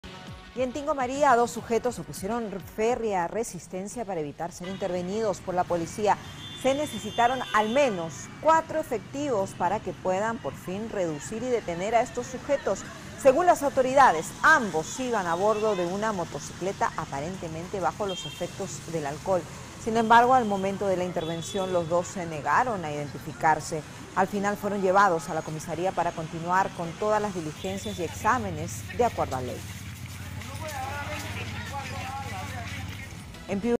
Tingo María: dos sujetos en presunto estado de ebriedad se resisten a ser intervenidos por la PNP
En Tingo María, dos sujetos opusieron férrea resistencia para evitar ser intervenidos por miembros de la policía en plena vía pública. Se necesitaron al menos de cuatro efectivos para que por fin puedan reducir y detener a los sujetos.

Según las autoridades, ambos iban a bordo de una motocicleta manejando, aparentemente, bajo los efectos del alcohol. Sin embargo, al momento de la intervención los dos se negaron a identificarse.

Al final fueron llevados a la comisaria para continuar con las diligencias y exámenes de acuerdo a ley.